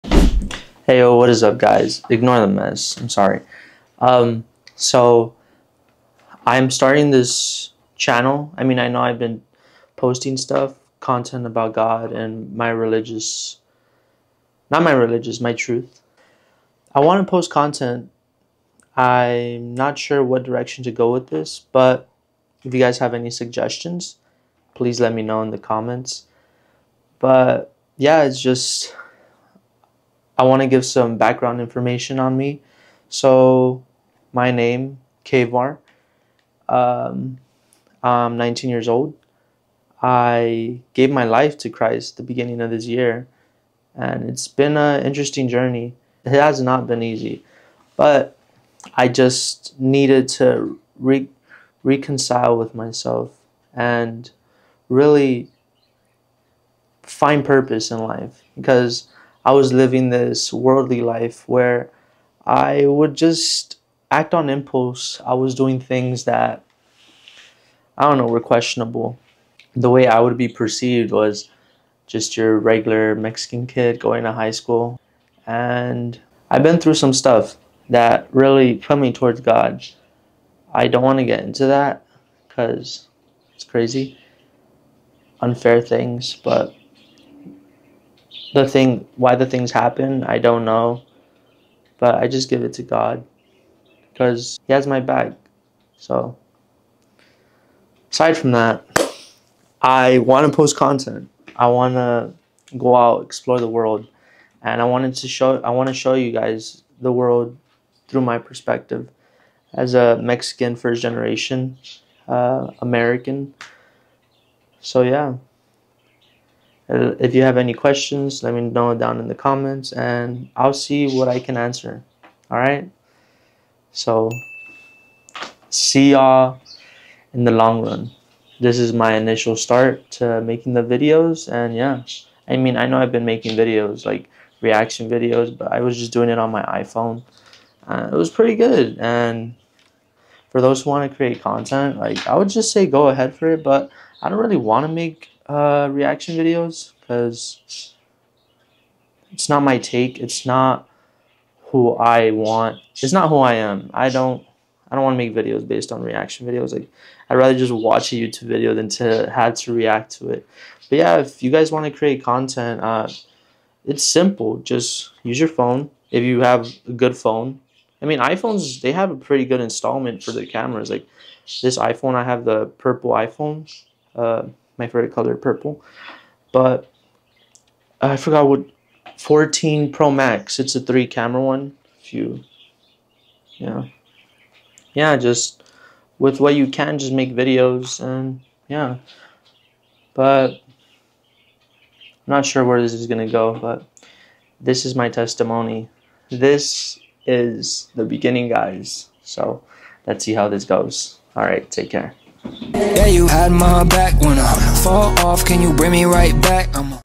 START MAKING CONTENT (My Story)
I am going to further my content creating. For those who have been following me, thank you, I really appreciate the support. This is honestly for you guys so let me know what you guys want to see.